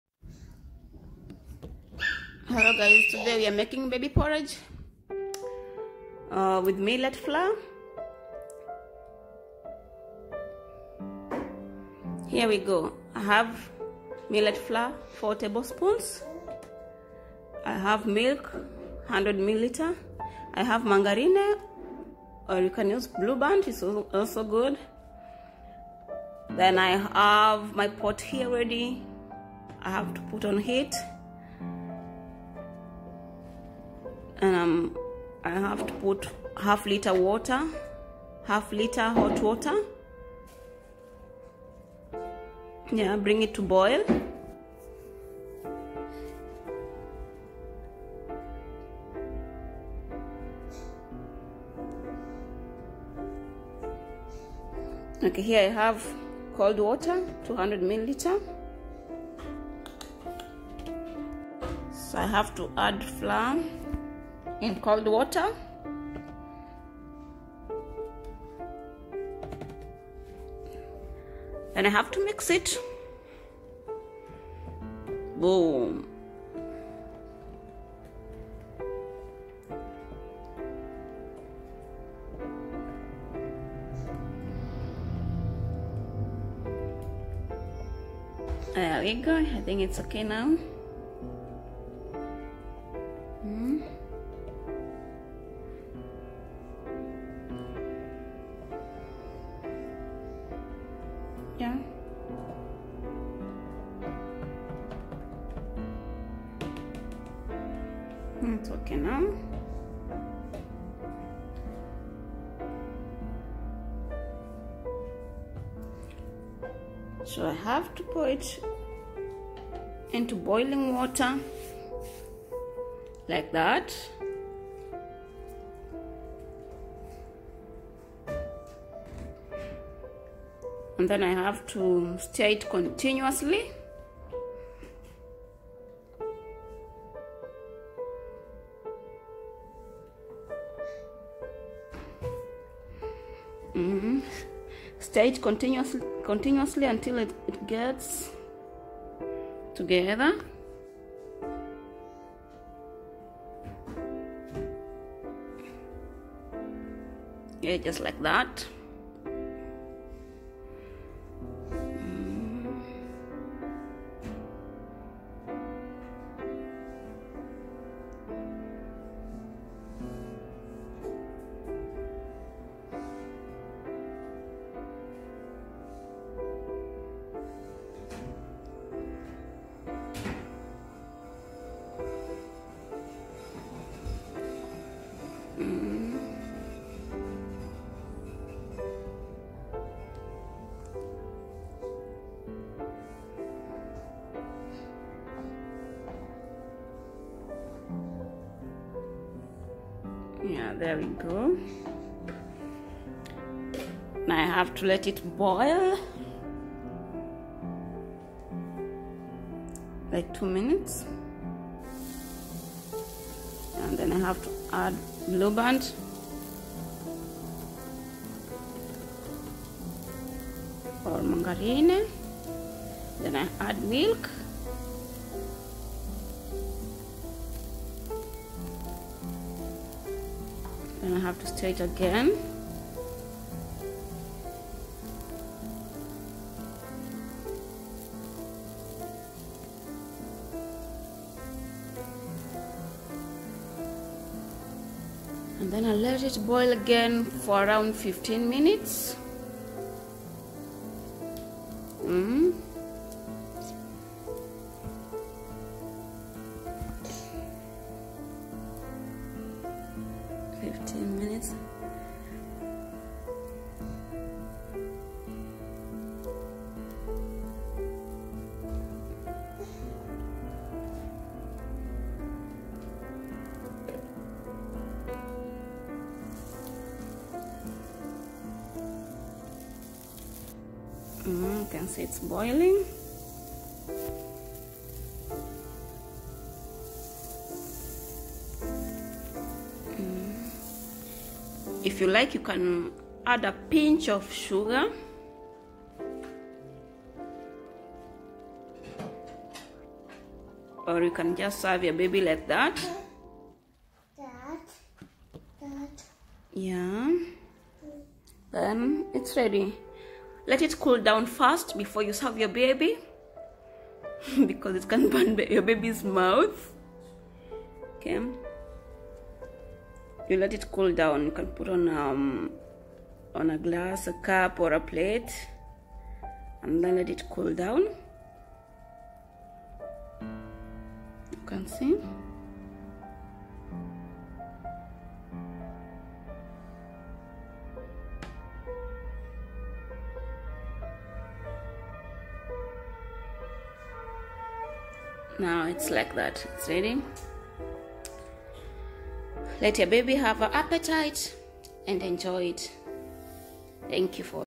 6-12 Months babies Appetizer || porridge || uji wa wimbi || baby food. 😘
Millet porridge is very healthy for babies and it provides appetite for babies.